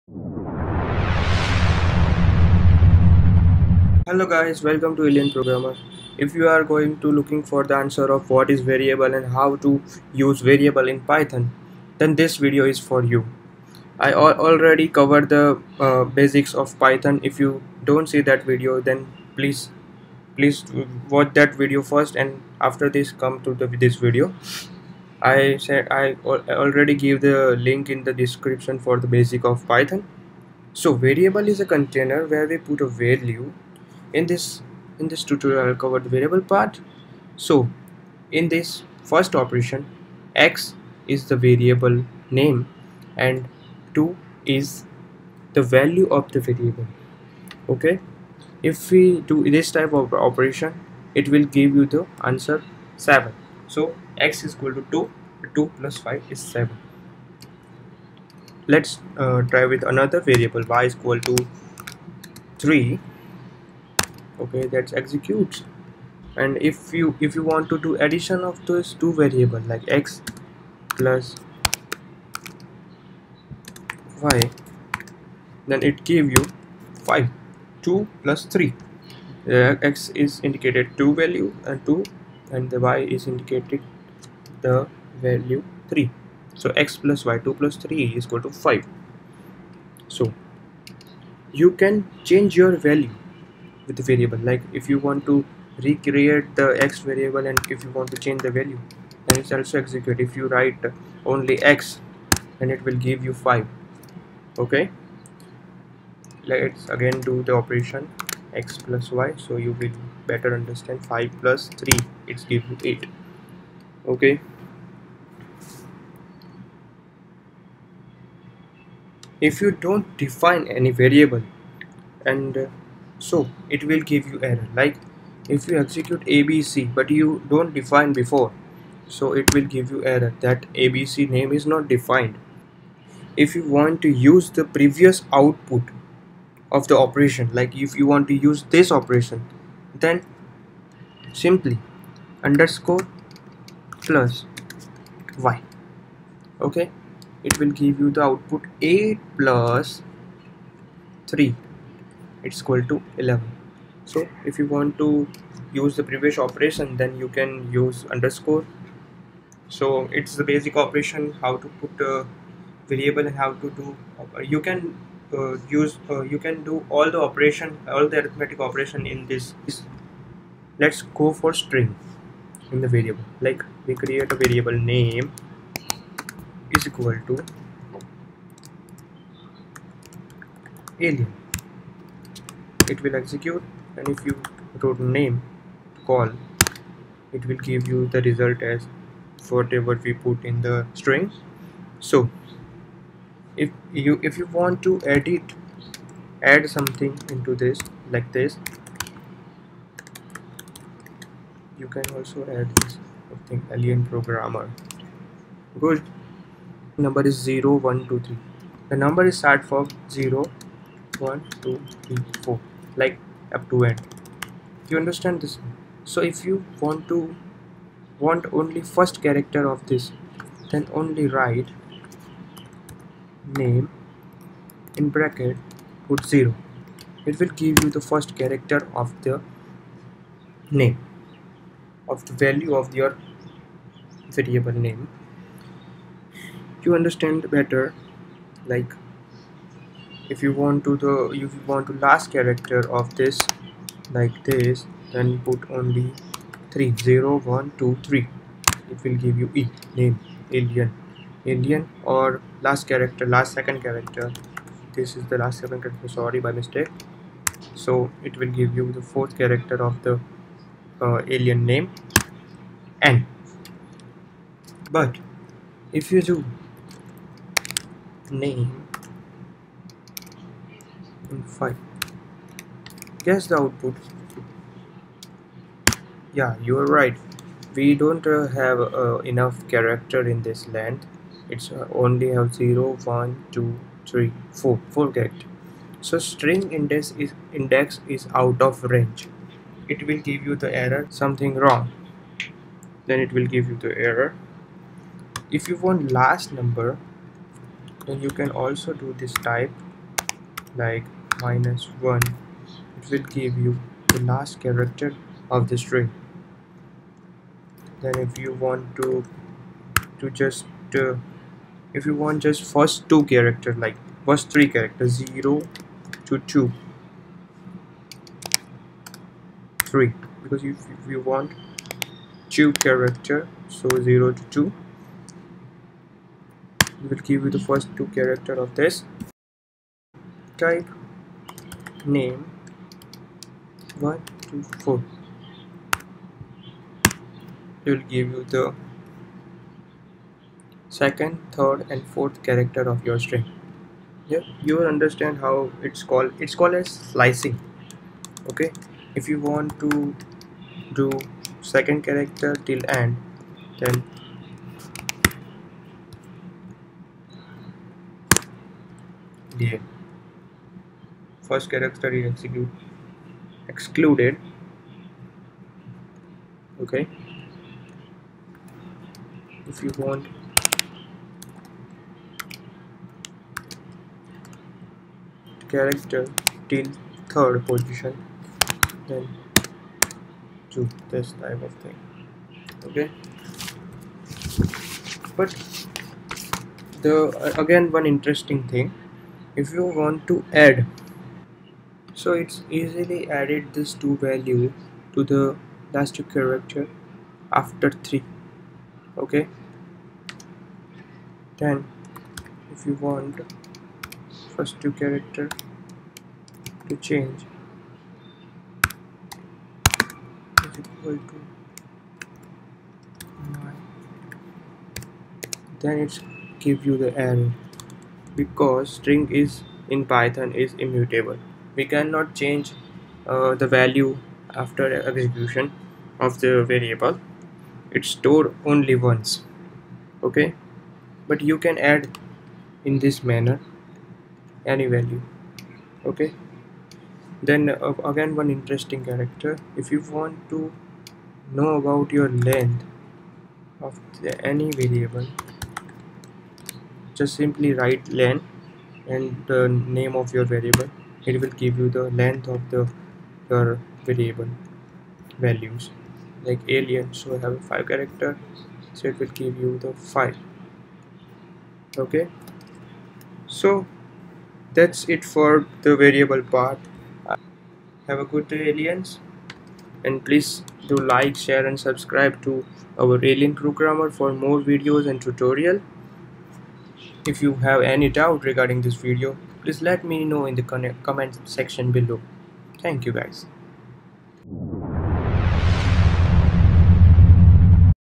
# 4 Variables In Python | Python in Spyder | Python Tutorial For Beginners
In this video we will see :
- What is variable
- Why do we need them
- How to assign value to variable
- Fetching value of previous operation
- String value to variable
- Fetching value of string variable by index number
- Finding length of string